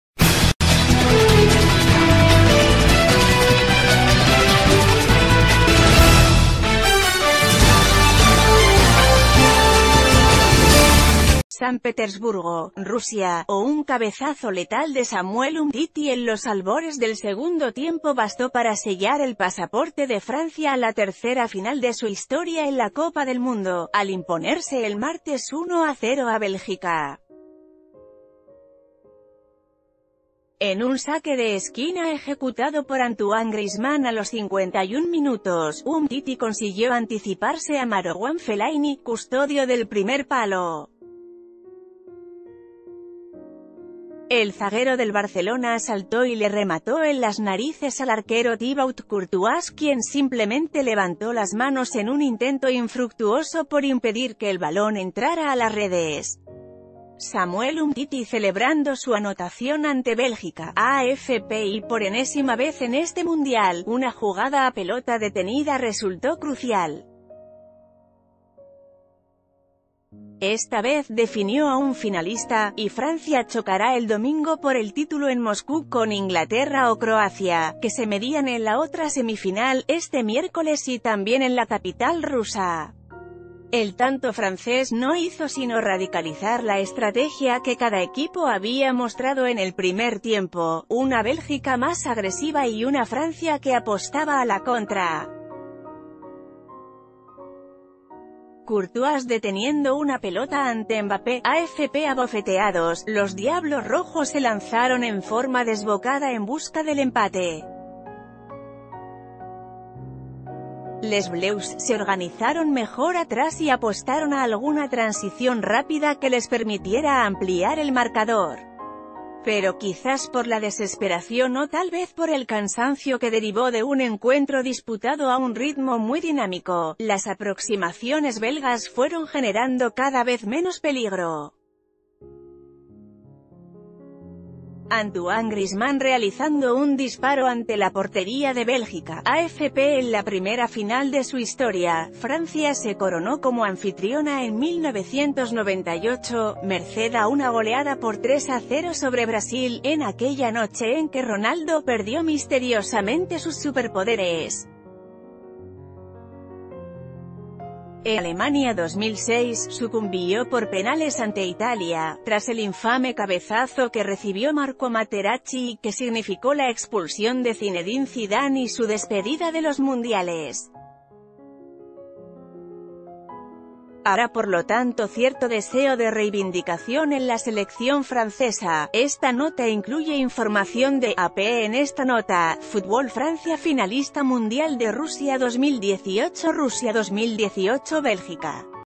Francia vence 1-0 a Bélgica y es el primer finalista del Mundial
San Petersburgo, Rusia.- Un cabezazo letal de Samuel Umtiti en los albores del segundo tiempo bastó para sellar el pasaporte de Francia a la tercera final de su historia en la Copa del Mundo, al imponerse el martes 1-0 a Bélgica. En un saque de esquina ejecutado por Antoine Griezmann a los 51 minuto...